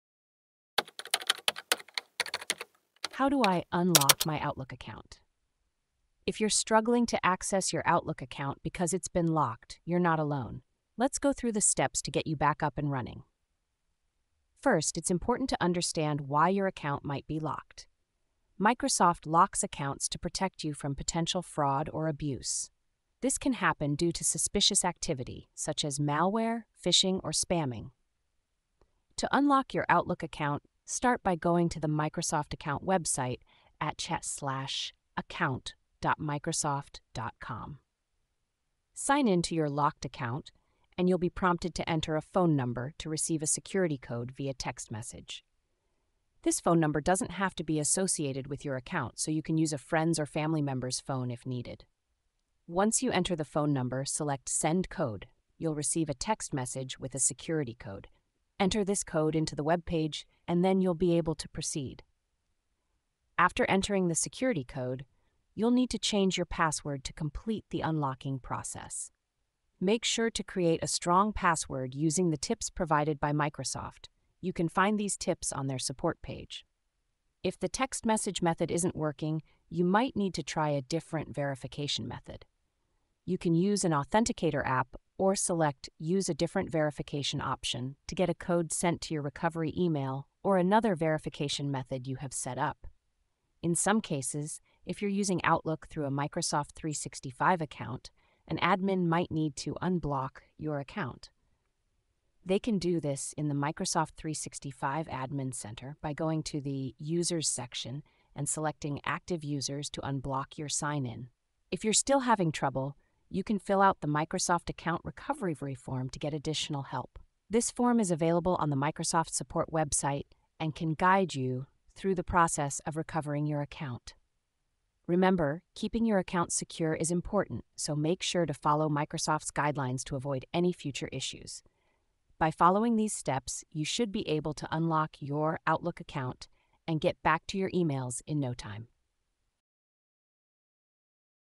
How Do I Unlock My Outlook Account? - TheEmailToolbox.com
How Do I Unlock My Outlook Account? Are you having trouble accessing your Outlook account? In this detailed video, we will guide you through the process of unlocking your locked Outlook account. We’ll begin by discussing the reasons why Microsoft may lock your account, highlighting the importance of security measures in place to protect your information. You'll learn how to navigate the Microsoft account website and the steps needed to verify your identity using a security code sent via text message.

Additionally, we will cover alternative verification methods, including the use of an authenticator app and recovery email options. For those using Outlook through a Microsoft 365 account, we will explain the role of administrators in the unlocking process. If you continue to face challenges, we will provide information on completing the Microsoft account recovery form for further assistance.

Maintaining the security of your account is essential, and we’ll share tips on how to create a strong password to safeguard your information moving forward. Join us in this informative session to regain access to your Outlook account and ensure your emails are secure.